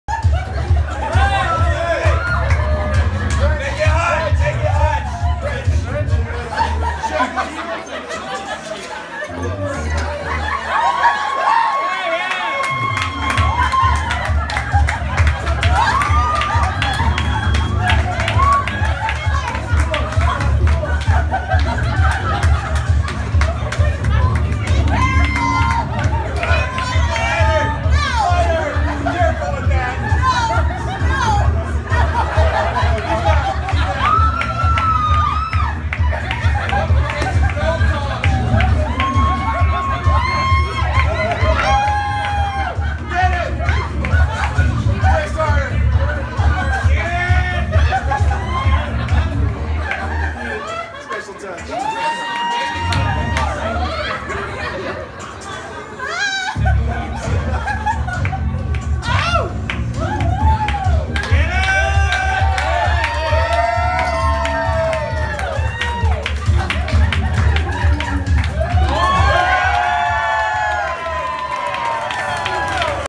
Garter Belt Surprise Dance
My husband surprised me with a hilarious garter belt dance involving his groomsmen, tools and a torch.